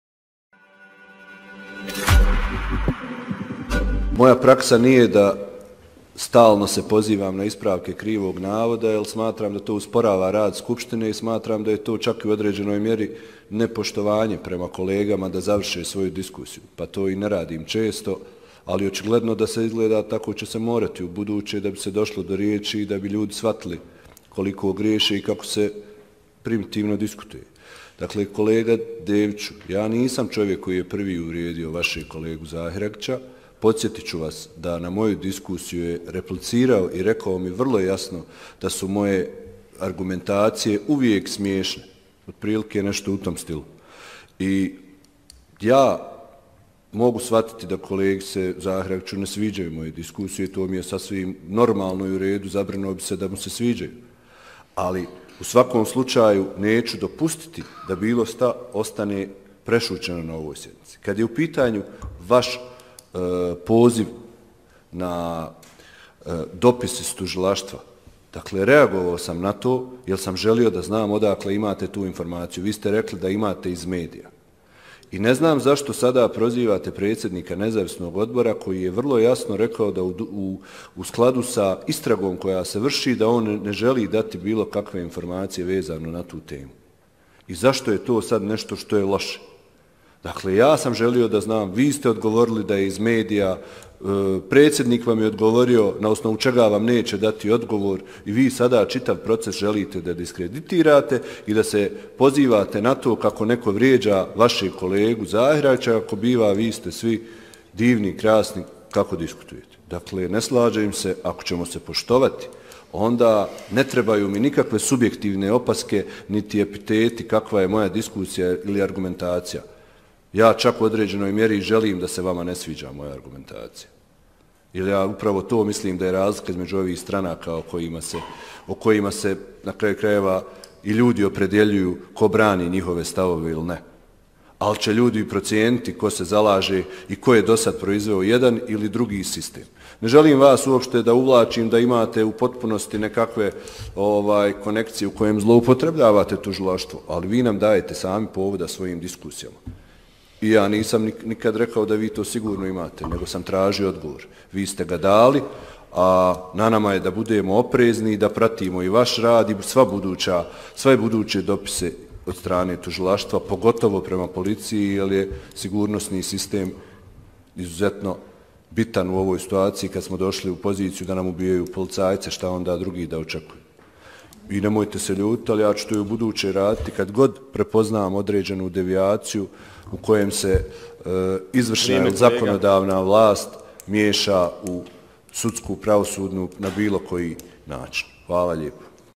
Stojanović: Kada nekažnjeno ubijaju policajce, šta građani da očekuju?
Izlaganje Igora Stojanovića, šefa Kluba zastupnika SDP BiH u Skupštini Kantona Sarajevo